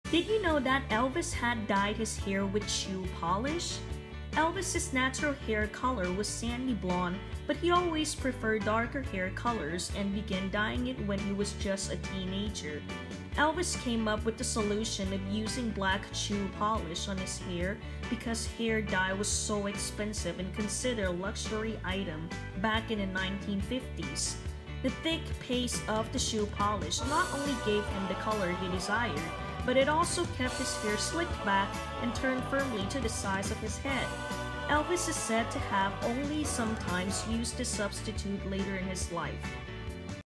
How Elvis dyed his hair 🧴
Elvis Presley dyed his hair using shoe polish when he was a teenager.